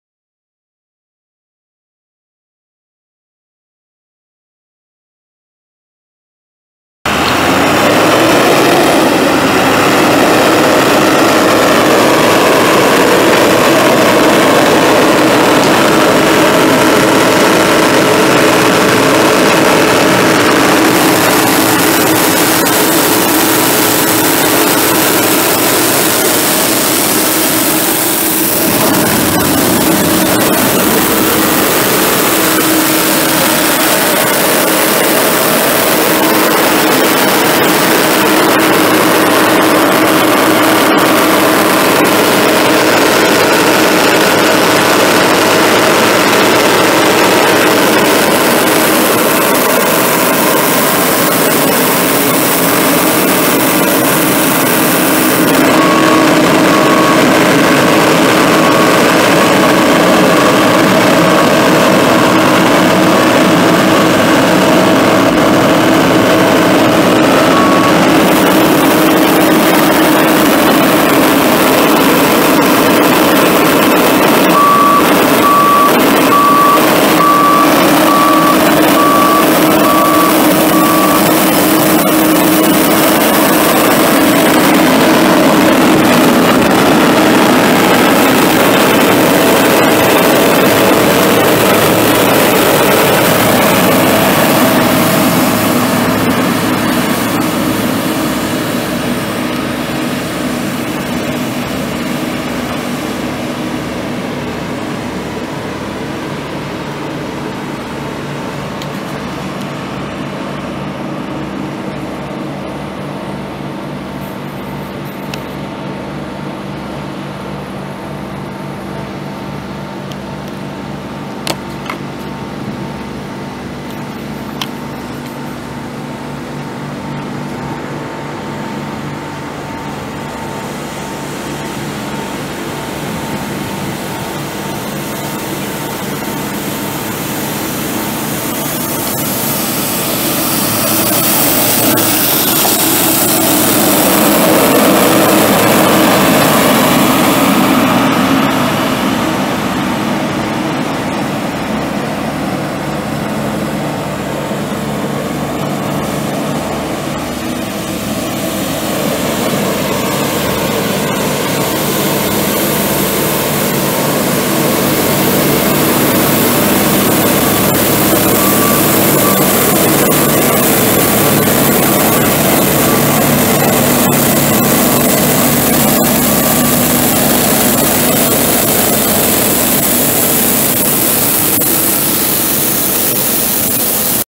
2005 Johnston 4000 sweeper for sale at auction | bidding closes May 3, 2018
2005 Johnston 4000 sweeper
11,186 miles on odometer
2,446 hours on meter
Cummins six cylinder turbo diesel engine
Automatic transmission
AC and heat
AM/FM/cassette
Suspension seat
Instructor seat
Backup alarm
Wipers
Work lights
Block heater
60"W broom
Adjustable broom pressure
35" dual gutter brooms
Gutter broom spray nozzles
Ecco Safety Director light control panel
Exterior visor
Spring suspension
10R22.5 front tires
315/80R22.5 rear tires
Manuals
Additional belt
Water components disconnected
Unit# 210